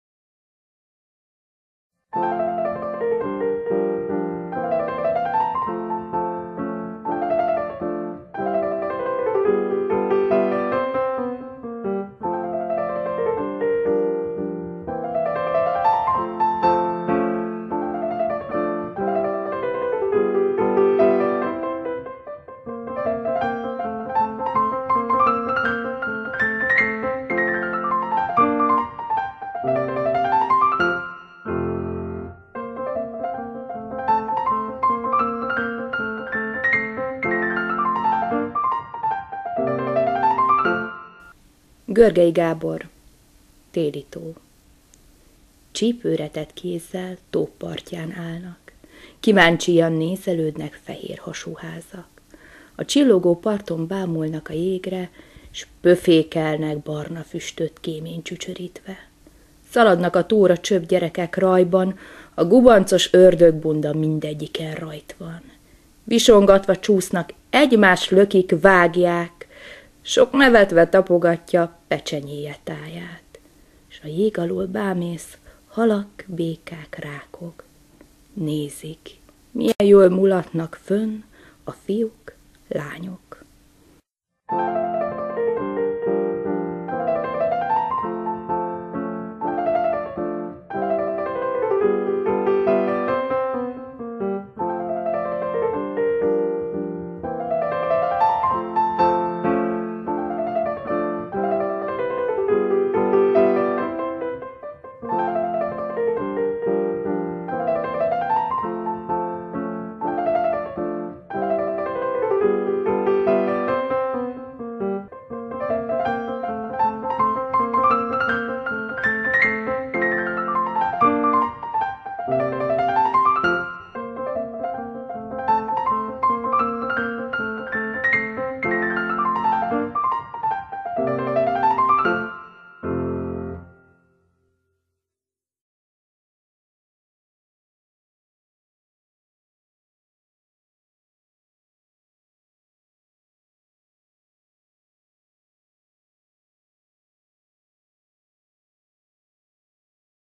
Korcsolyapálya a Balatonon
Hivatalosan is megnyitott a Balaton első korcsolyapályája Siófokon (2017)